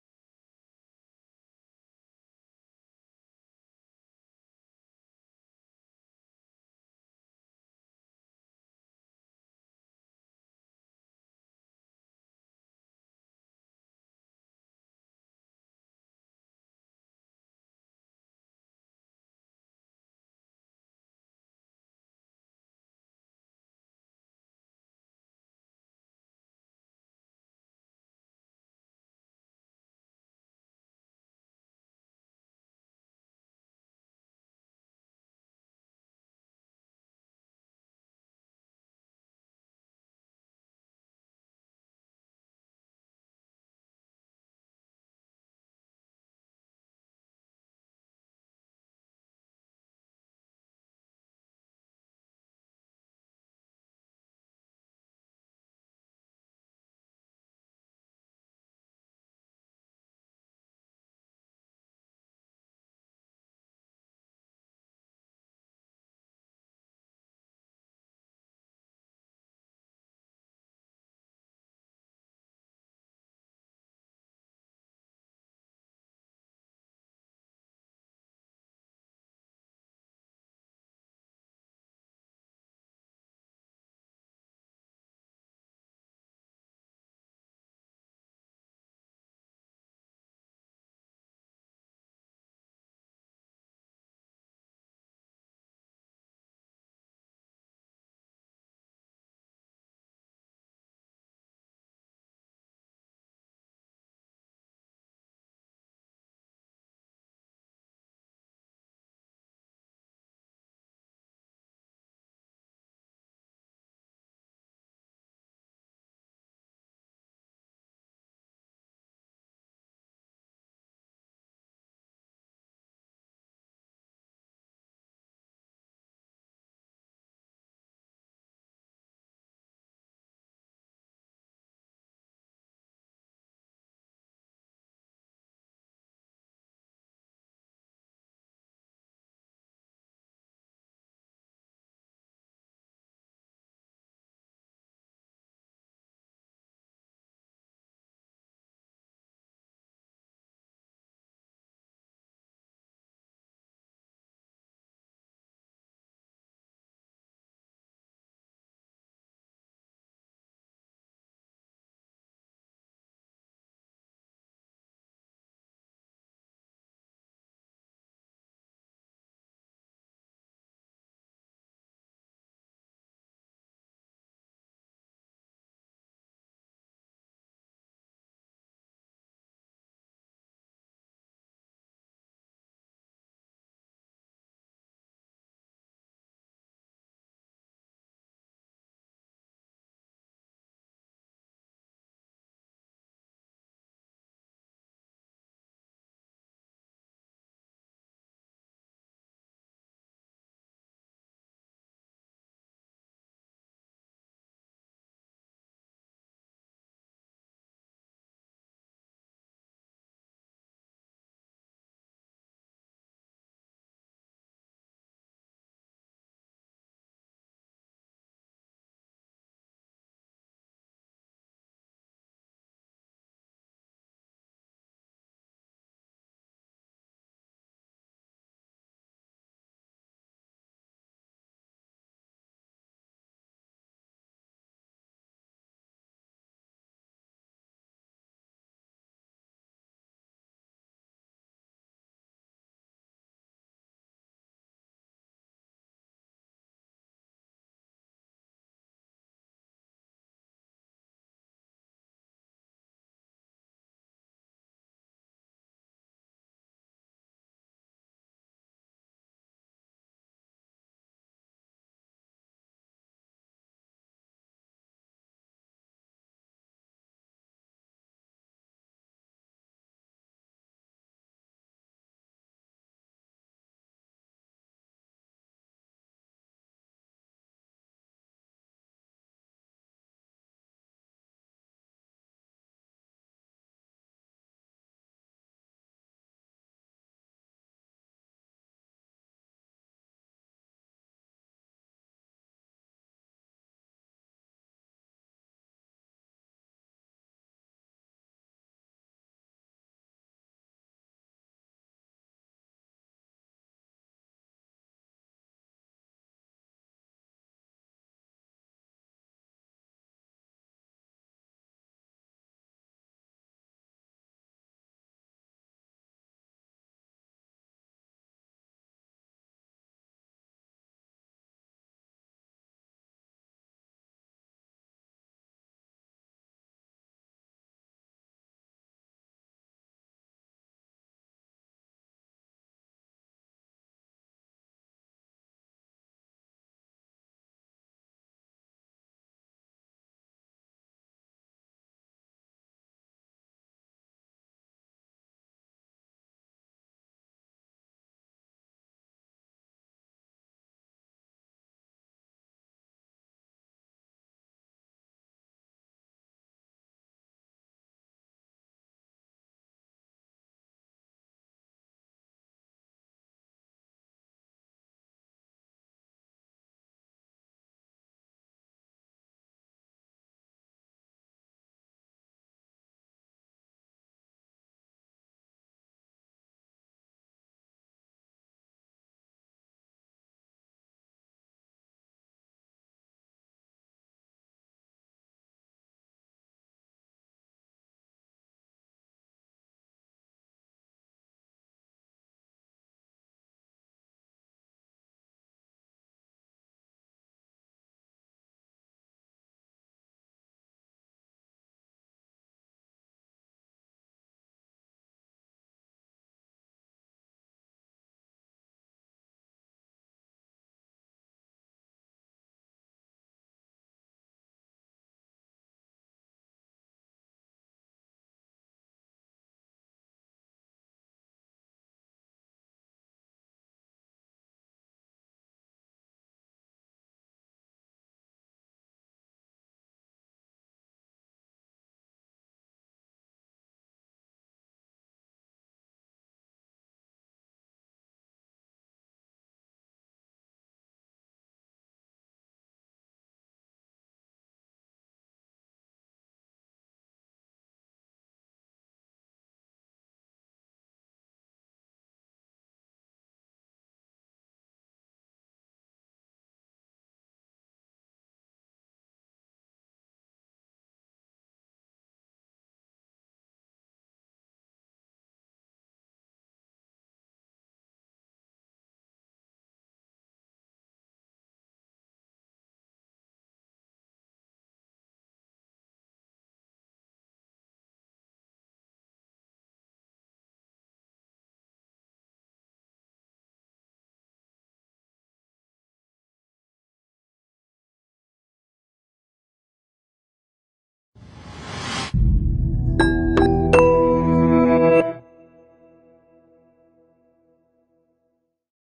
BLUM - jak zpracovat kování BLUM - stroj MINIPRESS M
Základní manuální vrtací stroj firmy BLUM pro zpracování závěsů, montážních podložek a vrtání řady otvorů v rastru 32 mm (pro podpěry polic).
Více informací na nebo u Vašeho obchodního partnera - prodejce sortimentu BLUM.